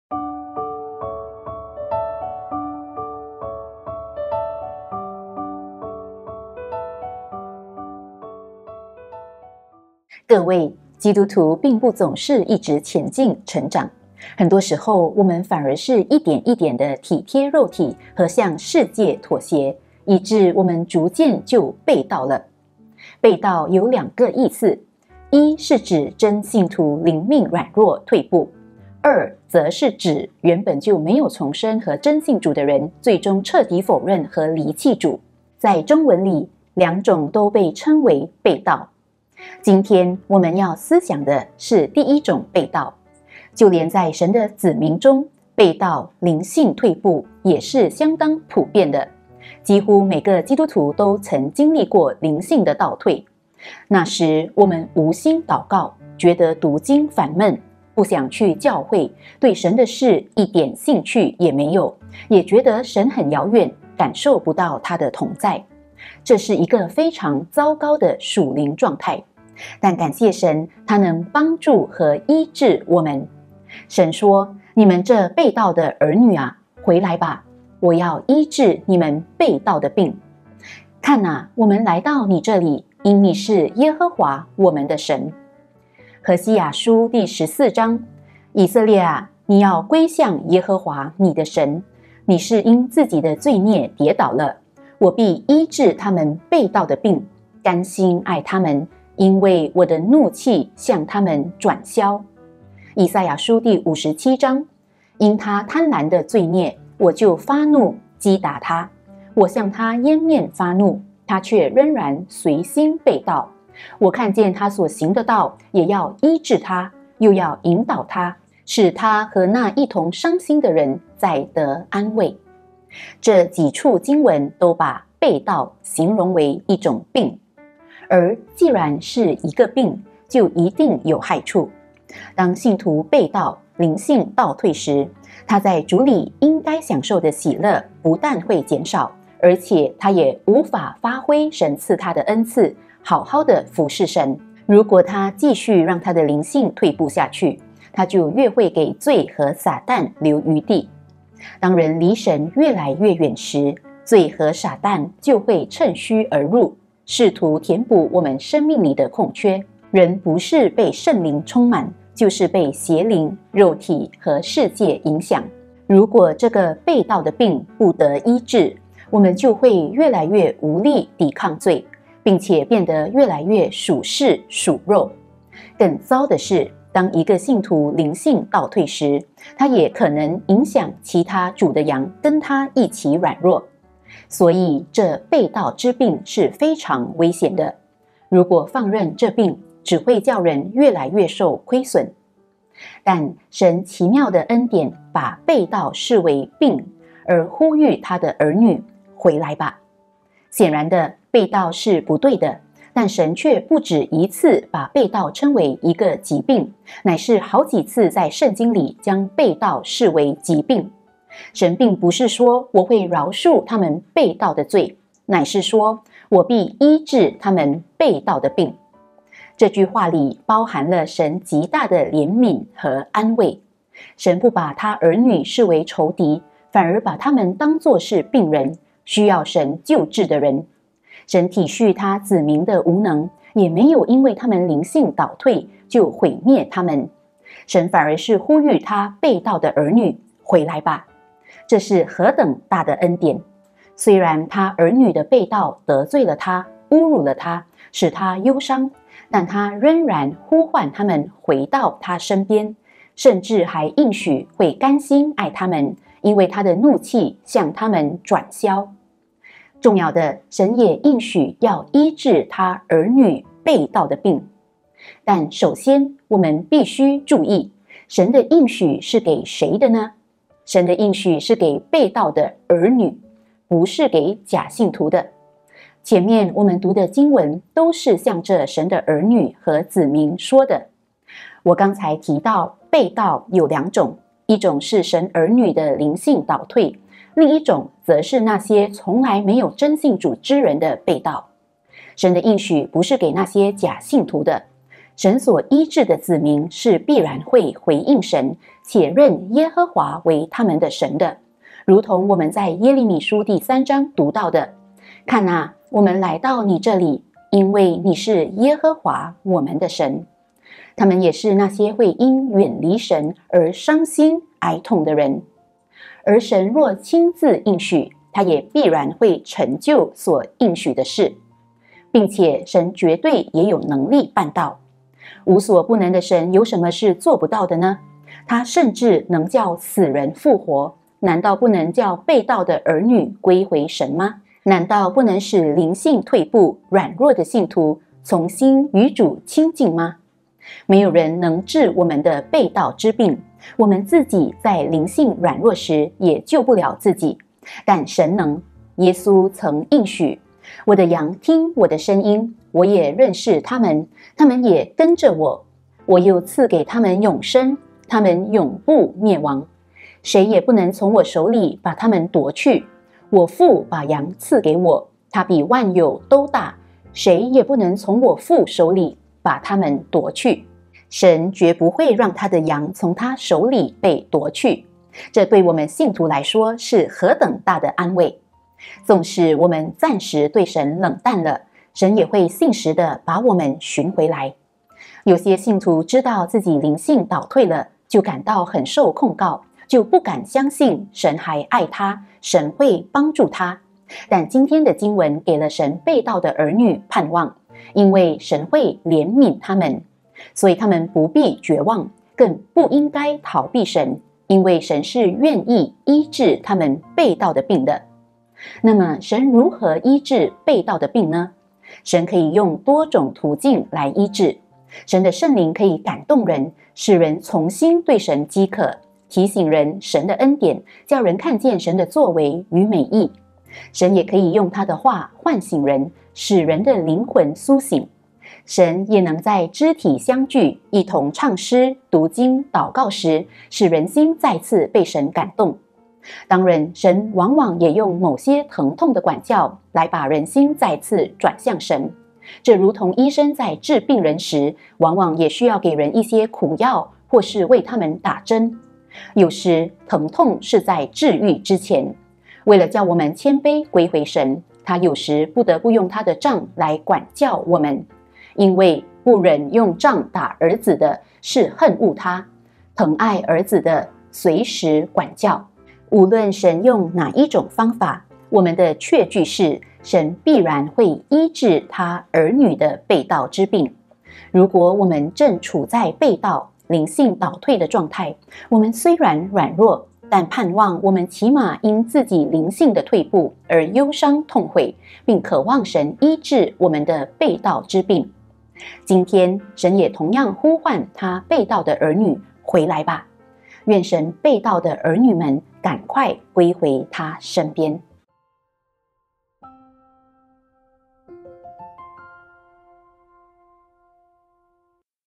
“我要医治你们背道的病”｜每日 - 20231204
主题：“我要医治你们背道的病”
经文：耶3:22; 何14:1, 4; 赛57:17-18; 约10:27-29; 箴13:24
分享者：林慧君传道
简介：就连在神的子民中，背道（灵性退步）也是相当普遍的。几乎每一个基督徒都曾经历过灵性的倒退。那时，我们无心祷告，觉得读经烦闷，不想去教会，对神的事一点兴趣也没有，也觉得神很遥远，感受不到祂的同在。这是一个非常糟糕的属灵状态。但感谢神！祂应许祂会医治祂儿女“背道的病”。

基督徒并不总是一直前进、成长的。很多时候，我们反而是一点一点的体贴肉体和向世界妥协，以致我们逐渐就背道了。“背道”有两个意思，一是指真信徒灵命软弱、退步；二则是指原本就没有重生和真信主的人最终彻底否认和离弃主。在中文里，两种都被称为“背道”。今天我们要思想的是第一种背道。就连在神的子民中，背道（灵性退步）也是相当普遍的。几乎每一个基督徒都曾经历过灵性的倒退。那时，我们无心祷告，觉得读经烦闷，不想去教会，对神的事一点兴趣也没有，也觉得神很遥远，感受不到祂的同在。这是一个非常糟糕的属灵状态。但感谢神，祂能帮助和医治我们。
神说：“你们这背道的儿女啊，回来吧！我要医治你们背道的病。’”“看哪！我们来到你这里，因你是耶和华我们的神。”《耶3:22》
《何14:1，4》以色列啊，你要归向耶和华你的神！你是因自己的罪孽跌倒了…“我必医治他们背道的病，甘心爱他们，因为我的怒气向他们转消。”
《赛57:17-18》因他贪婪的罪孽，我就发怒击打他。我向他掩面发怒，他却仍然随心背道。我看见他所行的道，也要医治他，又要引导他，使他和那一同伤心的人再得安慰。

1、 “背道的病”。
这几处经文都把“背道”形容为一种病。
1）既然是个“病”就一定有害处。
当信徒背道，灵性倒退时，他在主里应该享受的喜乐不但会减少，而且他也无法发挥神赐他的恩赐，好好地服侍神。如果他继续让他的灵性退步下去，他就越会给罪和撒旦留余地。当人离神越来越远时，罪和撒旦就会趁虚而入，试图填补我们生命里的空缺。人不是被圣灵充满就是被邪灵、肉体和世界影响。如果这个“背道的病”不得医治，我们就会越来越无力抵抗罪，并且变得越来越属世、属肉。更糟的是，当一个信徒灵性倒退时，他也可能影响其他主的羊跟他一起软弱。所以，这“背道之病”是非常危险的，如果放任这病，只会叫人越来越受亏损。

2）神奇妙的恩典把背道视为病而呼吁祂的儿女：“回来吧！”。
显然的，背道是不对的，但神却不止一次把背道称为一个疾病，乃是好几次在圣经里将背道视为疾病。神并不是说：“我会饶恕他们背道的罪”，乃是说：“我必医治他们背道的病”。这句话里包含了神极大的怜悯和安慰。神不把祂的儿女视为仇敌，反而把他们当作是病人，需要神救治的人。神体恤祂子民的无能，也没有因为他们灵性倒退就毁灭他们。神反而是呼吁祂背道的儿女：“回来吧！”。这是何等大的恩典！虽然祂儿女的背道得罪了祂，侮辱了祂，使祂忧伤，但祂仍然呼唤他们回到祂身边，甚至还应许会“甘心爱他们，因为祂的怒气向他们转消”。

2、 神要医治祂儿女背道的病。
1）神的应许是给背道的“儿女”，不是给假信徒的。
前面我们读的经文都是向着神的儿女和子民说的。我刚才提到“背道”有两种，一种是神儿女的灵性倒退；另一种则是那些从来没有真信主之人的背道。神的应许不是给那些假信徒的。神所医治的子民是必然会回应神，且认耶和华为他们的神的，如同我们在《耶3》读到的：“看哪！我们来到你这里，因你是耶和华我们的神。”他们也是那些会因远离神而“伤心”《赛57:18》、哀恸的人。

2）而神若亲自应许，祂也必然会成就所应许的事。
并且，神绝对也有能力办到。无所不能的神有什么是做不到的呢？祂甚至能叫死人复活，难道不能叫背道的儿女归回神吗？难道不能使灵性退步、软弱的信徒重新与主亲近吗？没有人能治我们的背道之病，我们自己在灵性软弱时也救不了自己。但神能！耶稣曾应许：“我的羊听我的声音，我也认识他们，他们也跟着我。我又赐给他们永生，他们永不灭亡，谁也不能从我手里把他们夺去。我父把羊赐给我，他比万有都大，谁也不能从我父手里把他们夺去。”《约10:27-29》神绝不会让祂的羊从祂手里被夺去；这对我们信徒来说是何等大的安慰啊！纵使我们暂时对神冷淡了，神也会信实地把我们寻回来的。有些信徒知道自己灵性倒退了，就感到很受控告，就不敢相信神还爱他，神会帮助他。但今天的经文给了神背道的儿女们盼望，因为神会怜悯他们。所以他们不必绝望，更不应该逃避神，因为神是愿意医治他们背道的病的。

3）神如何医治背道的病呢？
神可以用多种途径来医治。神的圣灵可以感动人，使人重新对神饥渴，提醒人神的恩典，叫人看见神的作为与美意。神也可以用祂的话唤醒人，使人的灵魂苏醒。神也能在肢体相聚，一同唱诗、读经、祷告时，使人心再次被神感动。当然，神往往也用某些疼痛的管教来把人心再次转向神。这如同医生在治病人时，往往也需要给人一些苦药或是为他们打针。有时，疼痛是在治愈之前。为了叫我们谦卑归回神，祂有时不得不用祂的杖来管教我们。因为“不忍用杖打儿子的，是恨恶他；疼爱儿子的，随时管教。”《箴13:24》
无论神用哪一种方法，我们的确据是神必然会医治祂儿女的背道之病。如果我们正处在背道、灵性倒退的状态，我们虽然软弱但盼望我们起码因自己灵性的退步而忧伤痛悔，并渴望神医治我们的背道之病。今天，神也同样呼唤祂背道的儿女：“回来吧！”愿神背道的儿女们赶快归回祂身边！

欢迎您在苹果、谷歌应用商店，搜索并安装「基督生命」app，或上我们教会网站，来获取更多信息。